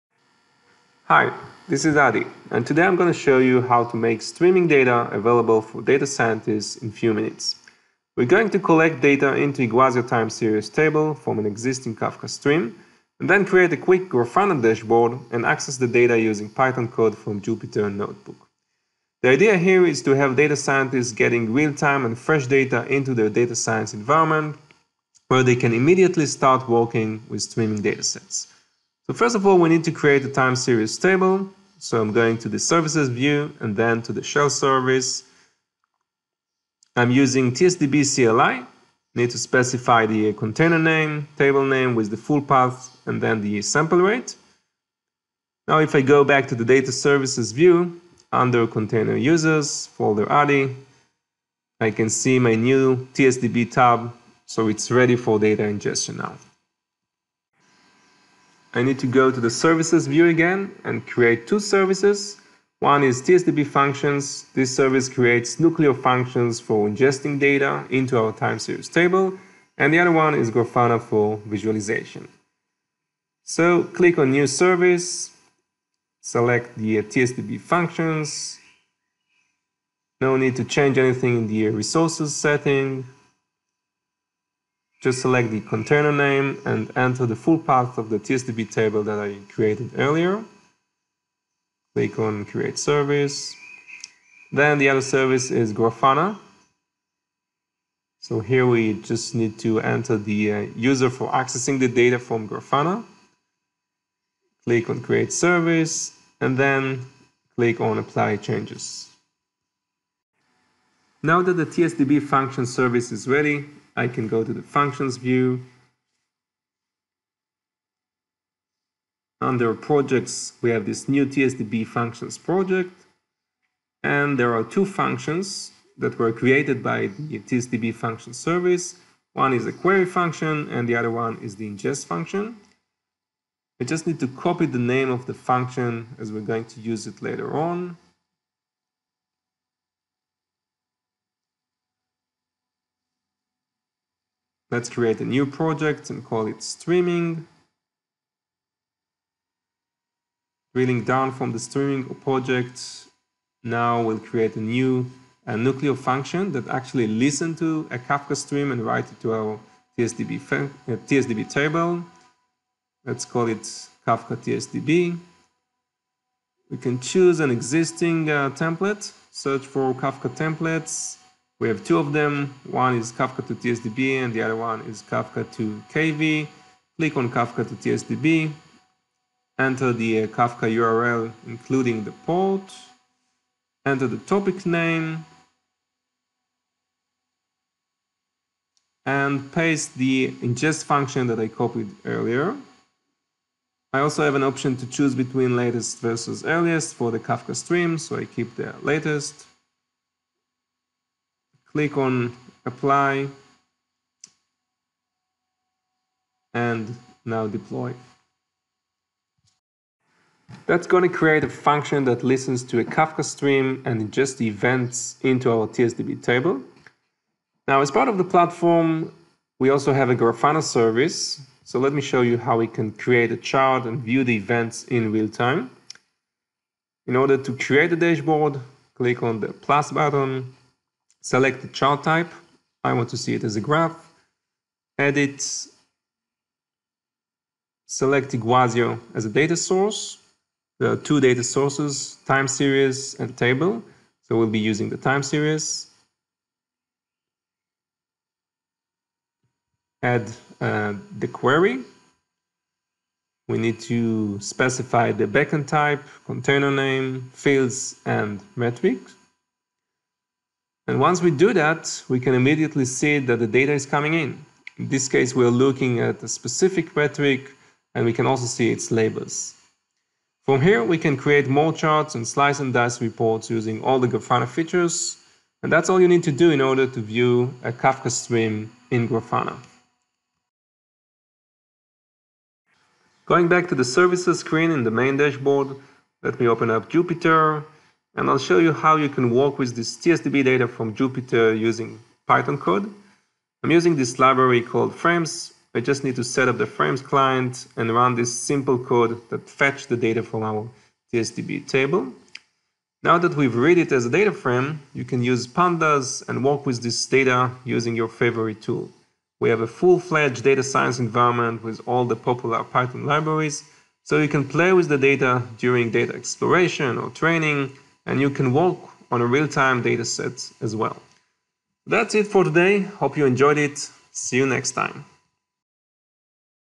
Real-time Streaming Into Your Data Science Environment
Iguazio's Director of Product Management, Adi Hirschtein, explains how to get fresh and real-time streaming data into a data science environment for exploration and visualization.